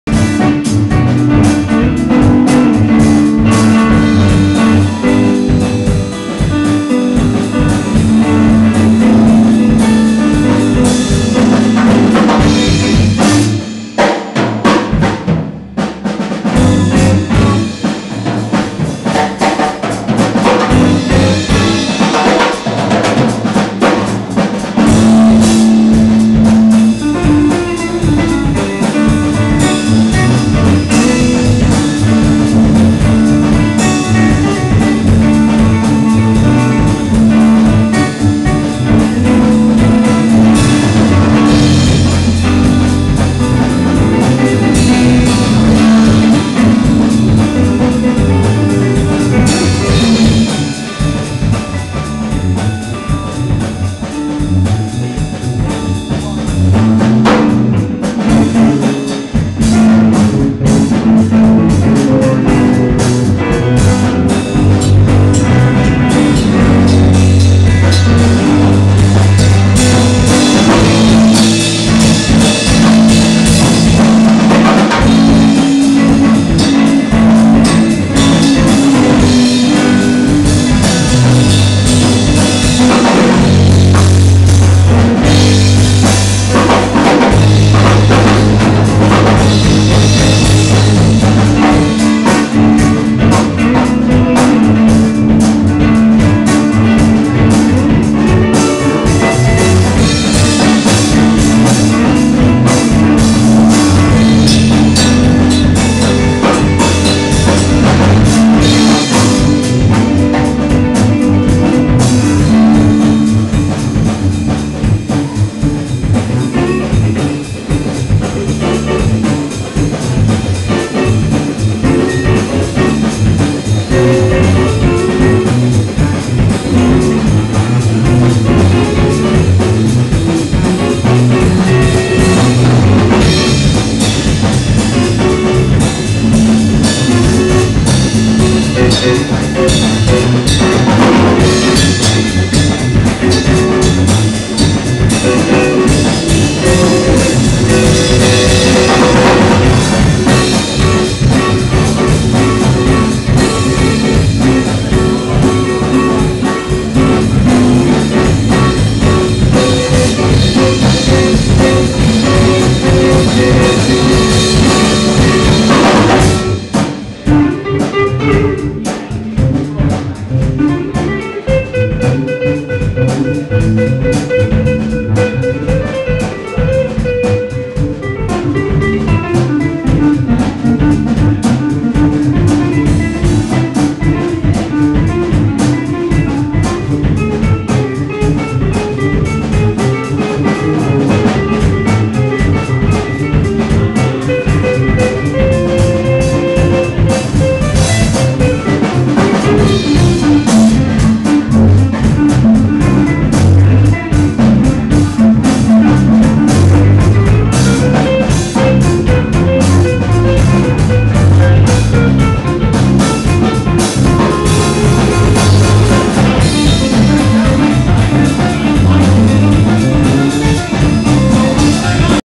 Verve Jazz Session
If you aint been coming to the verve in Terre Haute IN, on monday nights. Heres what youve been missing. - Recorded and uploaded on my iPhone 2g/3g with iCamcorder